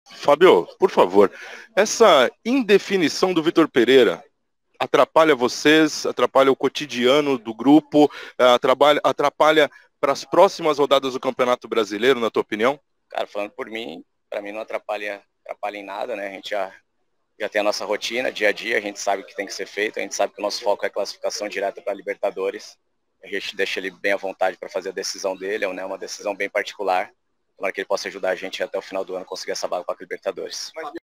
SERÁ MESMO? FÁBIO SANTOS RESPONDEU! - CORINTHIANS X FLUMINENSE
Direto da Zona Mista, Fábio Santos fala sobre a indecisão de Vítor Pereira sobre a temporada 2023

Identidade Corinthiana!

⚠️𝙉𝘼̃𝙊 𝘼𝘽𝙍𝘼!
𝙑𝘼́𝙇𝙄𝘿𝙊 𝙎𝙊𝙈𝙀𝙉𝙏𝙀 𝙋𝘼𝙍𝘼 𝙏𝙊𝙍𝘾𝙀𝘿𝙊𝙍 𝘾𝙊𝙍𝙄𝙉𝙏𝙃𝙄𝘼𝙉𝙊